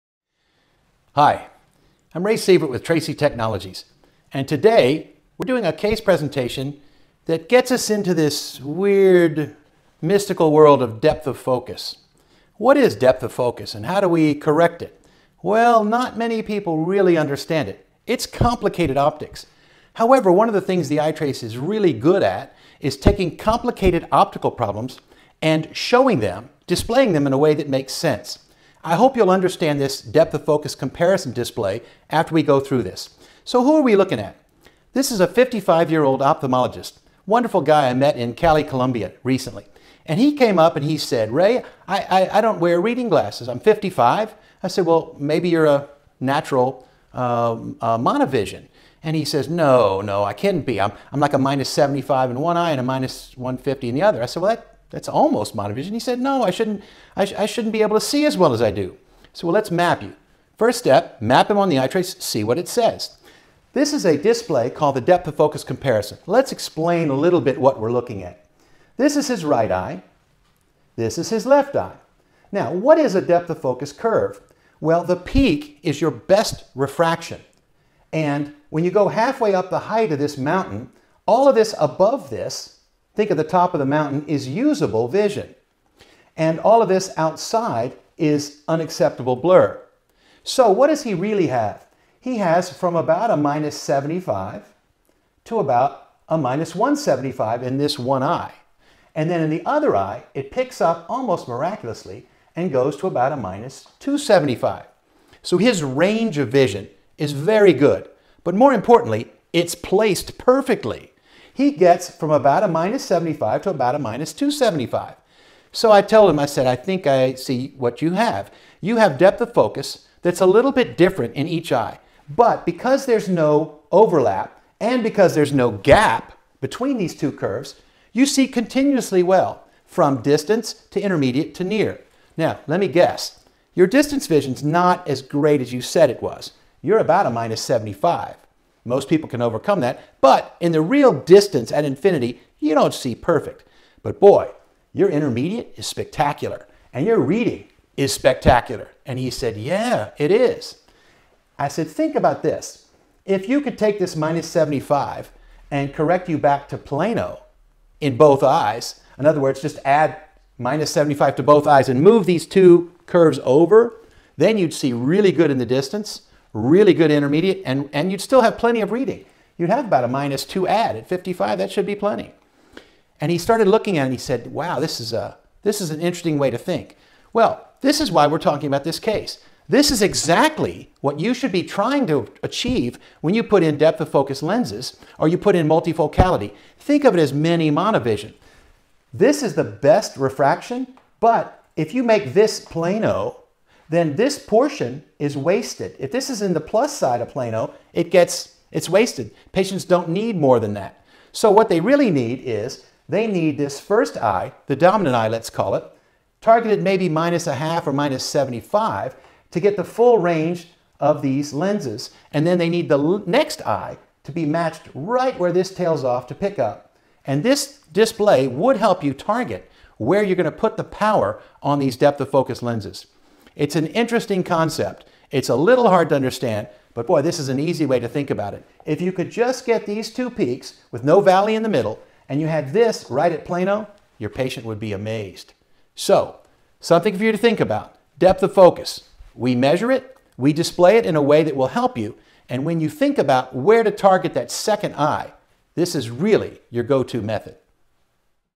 iTrace can help optimize refractive targets with Depth of Focus
The iTrace provides advanced displays for those who wish to evaluate the depth of focus in patient's eyes, particularly useful in studying presbyopic treatments to optimize refractive targets.